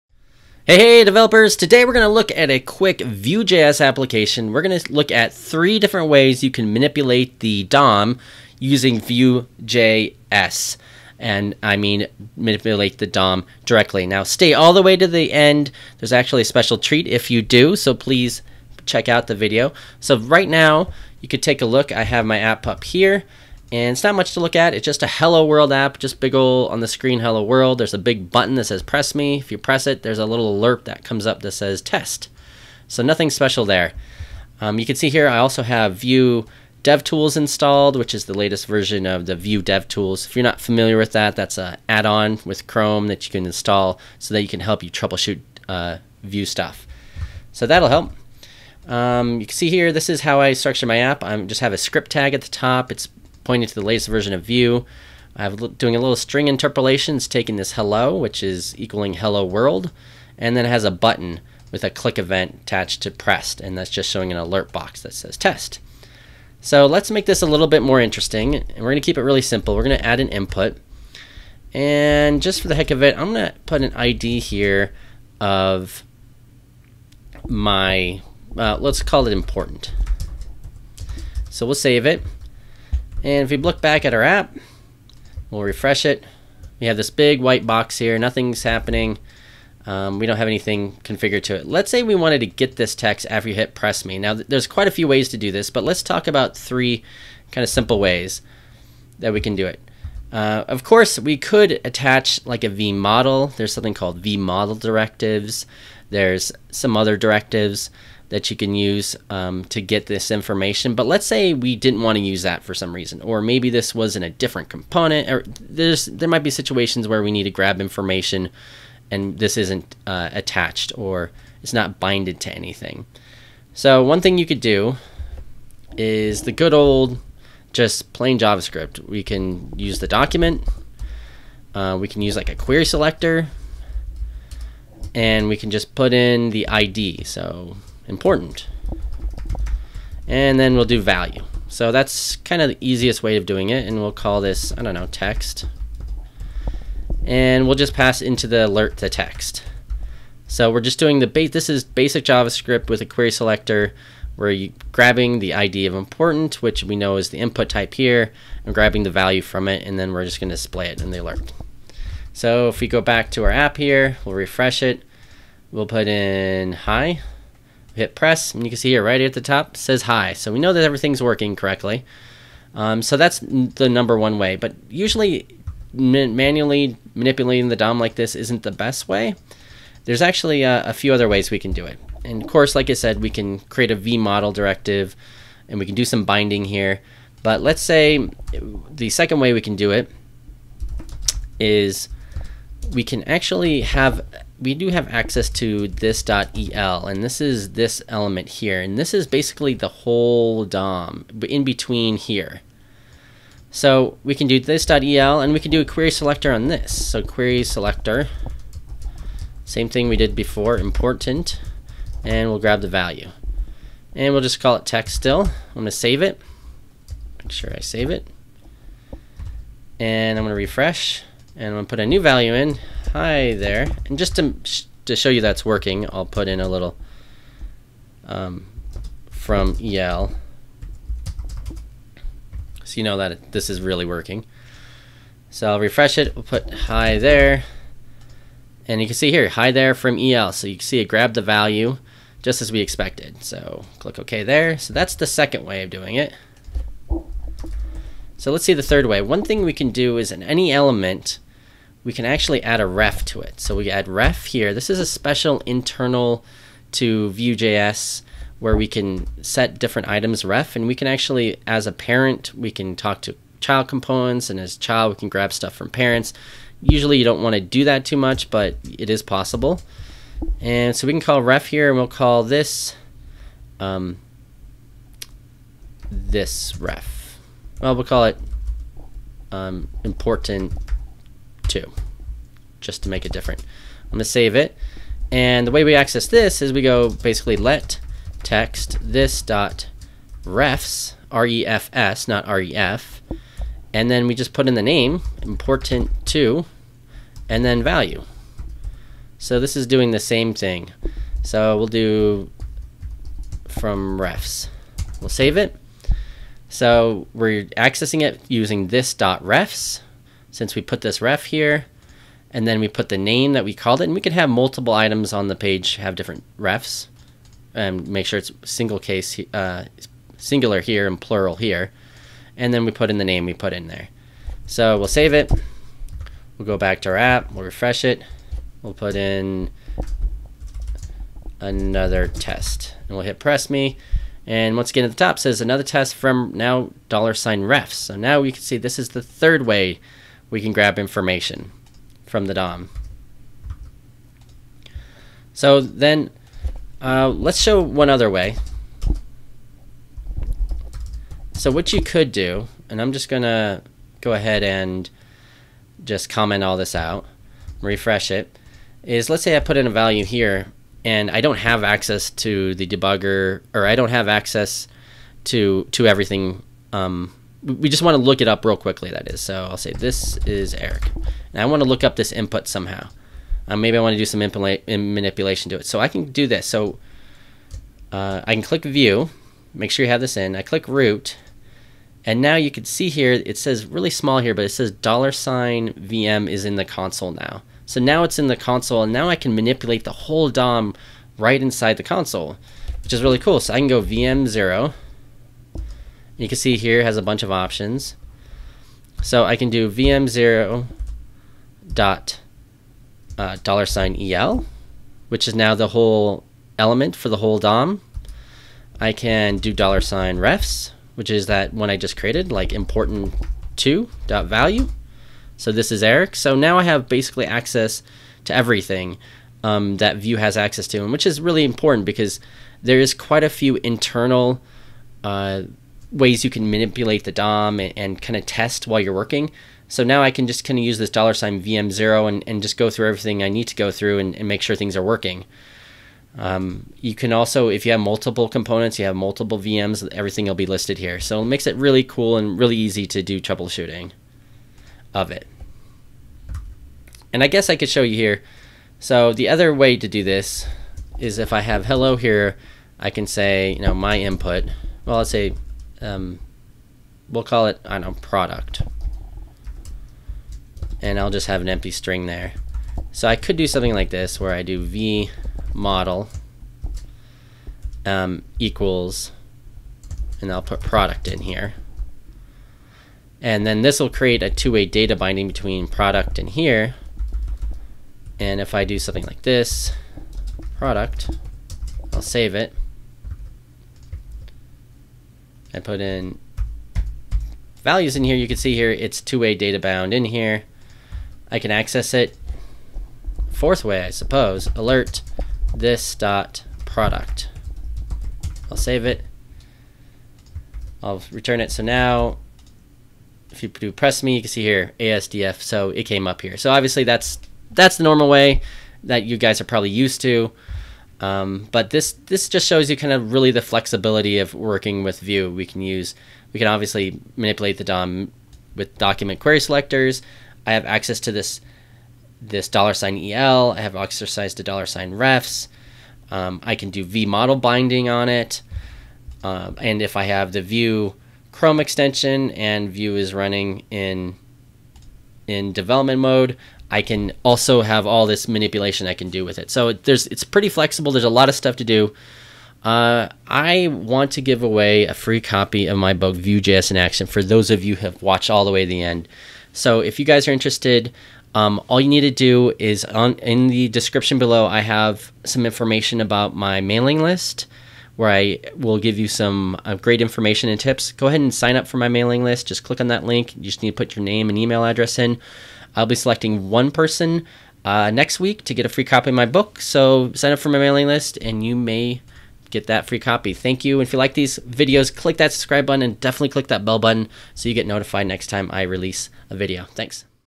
THREE TIPS FOR NEW VUE.JS DEVELOPERS MANIPULATING THE DOM
Heare are 3 tips for manipulating the DOM with Vue.js! In this video we look at this.$el, this.$refs and v-model! Vue.js is an amazing front-end framework that uses JavaScript. It's gaining
a lot of momentum! Watch all the way to the end for a surprise!

GET THE FIRST CHAPTER OF MY NEW BOOK Vue.js in Action!

GET MY LAST BOOK EMBER.JS COOKBOOK!

Links